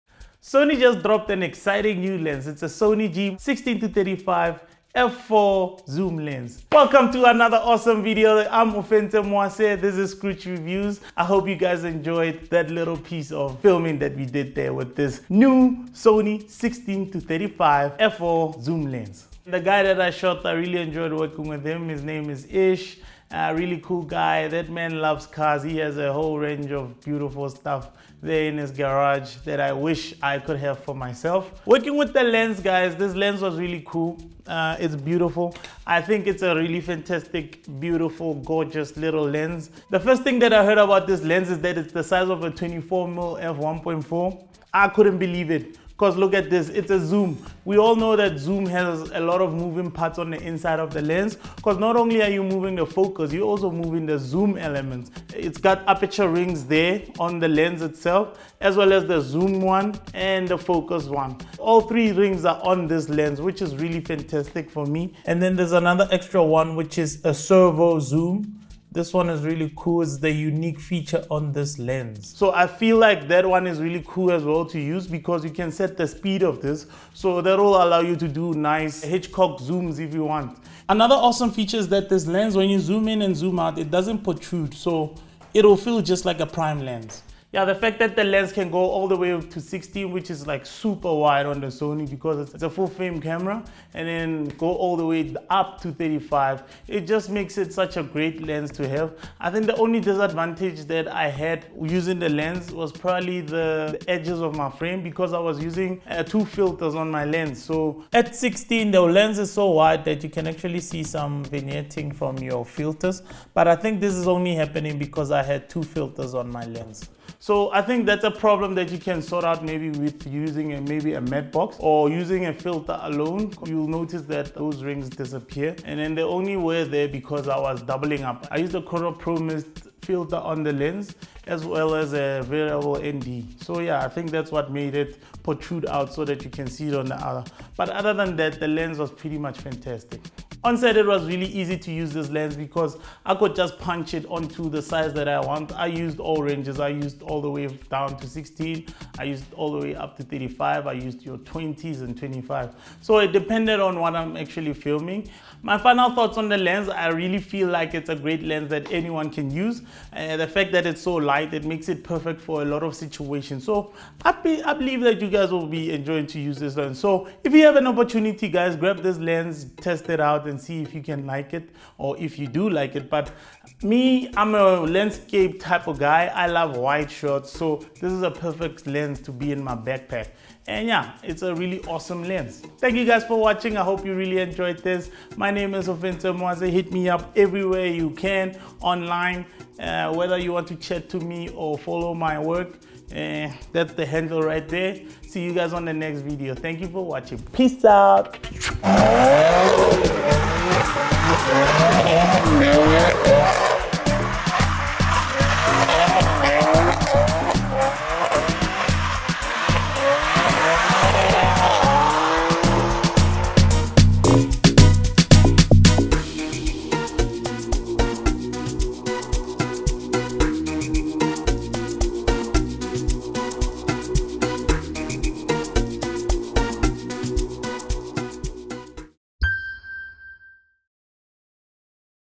Power Meets Passion | New Sony FE 4/PZ 16-35mm G-lens Review
Behind the Scenes look at Power Meets Passion short film shot on New Sony 16-35 mm F4 G-lens.

CREW
DOP/Director: Ofentse Mwase
Producer: Nelisiwe Mwase
LCamera Assistants: Bannz Version, Karabo Tshoagong
Editor: Ofentse Mwase
VFX: OMFilms
BTS Shooter: Cecil Karabo Tshoagong

Scene List
00:00 Intro
00:10 Power meets Passion
00:19 Meet Ish
00:31 Form Factor
01:39 Disadvantages
02:20 Usage
03:35 Final thoughts
03:02 Outro

Lens: Sony 24-70mm F2.8 G-Master
Tilta Matte Box
Neewer Shoulder Rig
Neewer Glidecam
GALAXY Stedicam Vest
Filter: Black ProMist 1/2 82mm
Gear: DJI Ronin-S
Hoya Variable ND Filter

Sony FX3 4K Footage | South Africa | Review